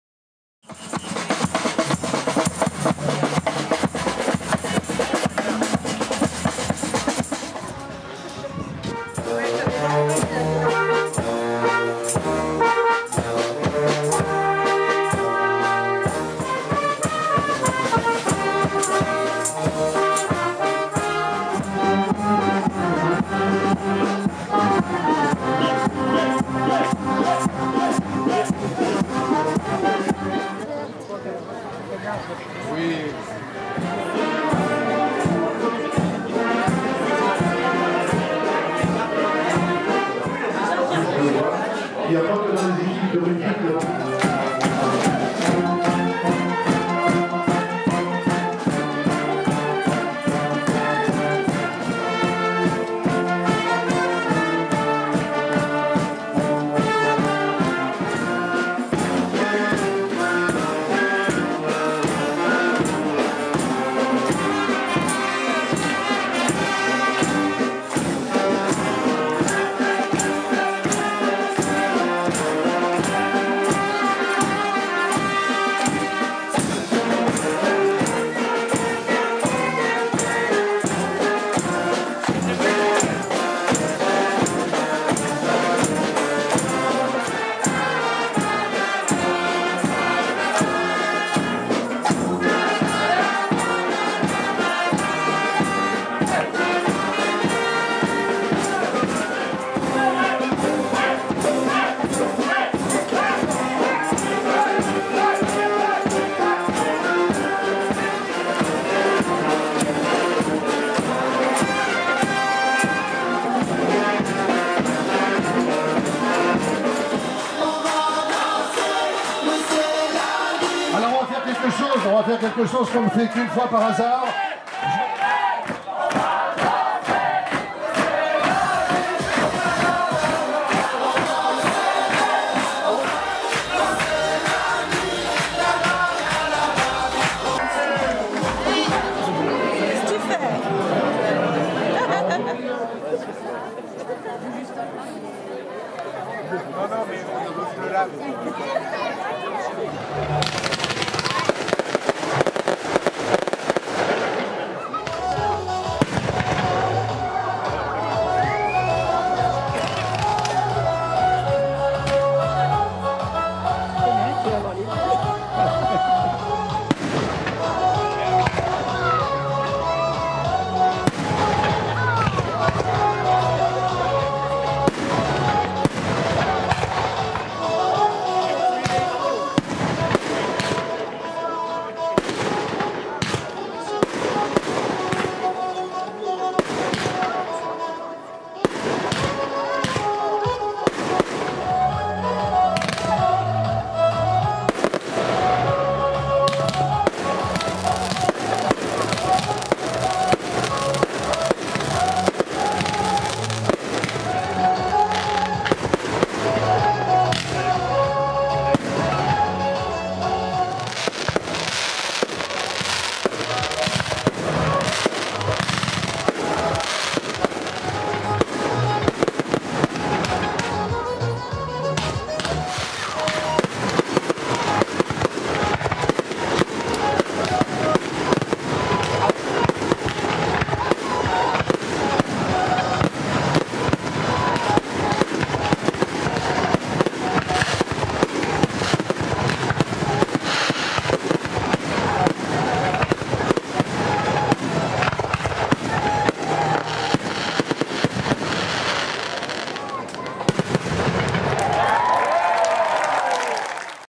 July 14th. the Firework Celebrations at Fresselines.
Join the fun and party at a typical fireworks celebration in France. Fresselines in the Creuse.